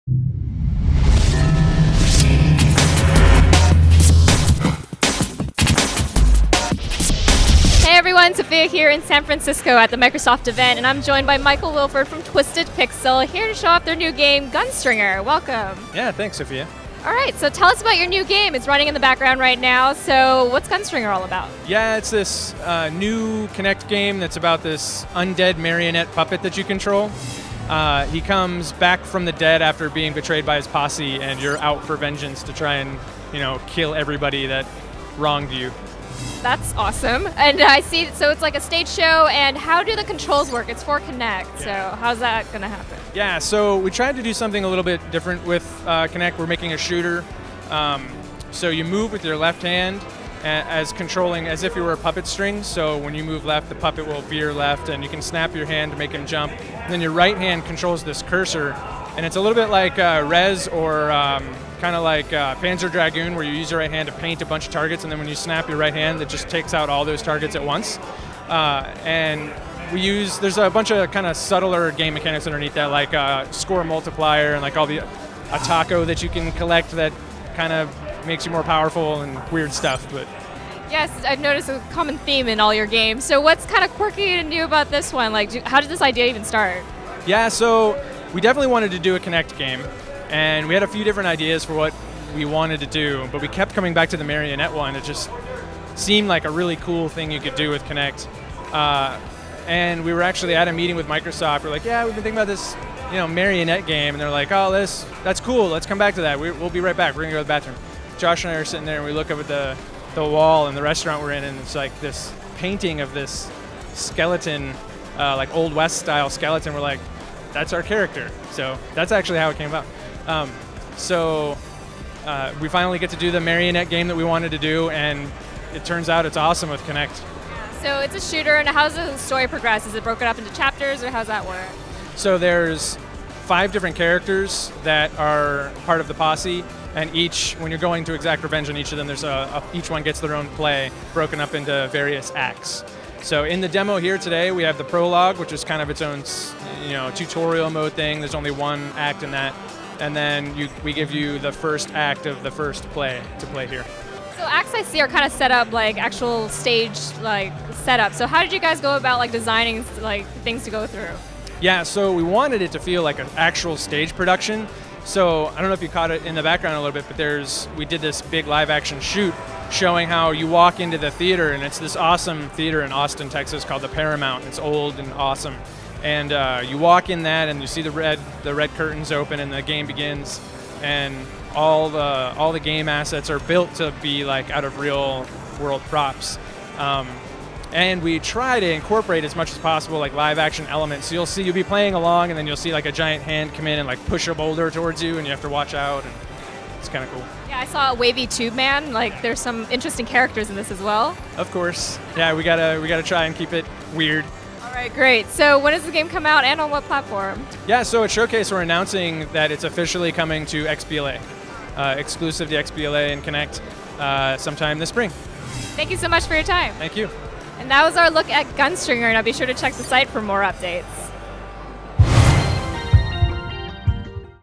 The Gunstringer Michael Wilford Interview
Sophia Tong talks to Michael Wolford about an undead marionette in this inteview for The Gunslinger.